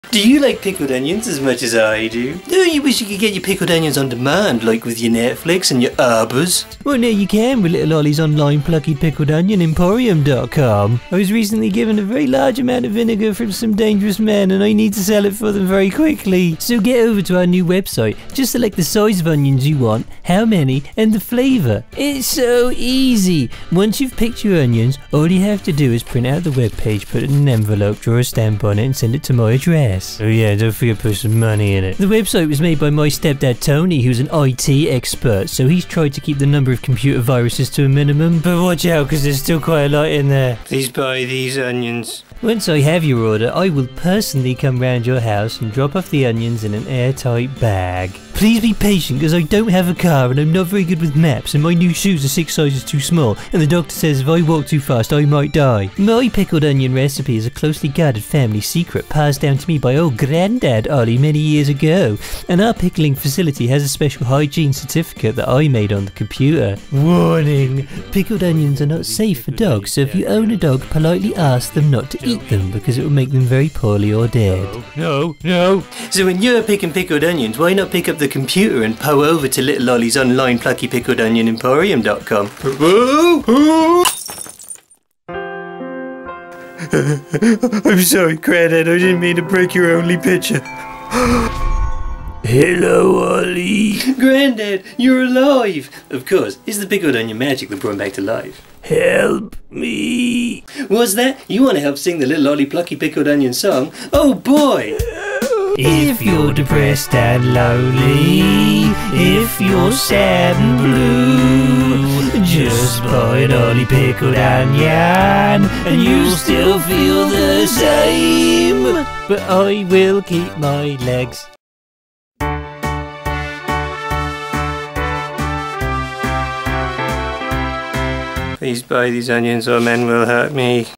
Please buy my pickled onions.
Why ahoy there and welcome to Little Ollie's Online Plucky Pickled Onion Emporium (Dot Com)! Ollie has all kinds of flavours for sale (well, three if you include the one where he sucks the vinegar off for you). He needs to sell them very, very quickly so head on over there and buy several hundred today! He hasn't actually figured out how web domains work so you'll have to go to his house to see the website.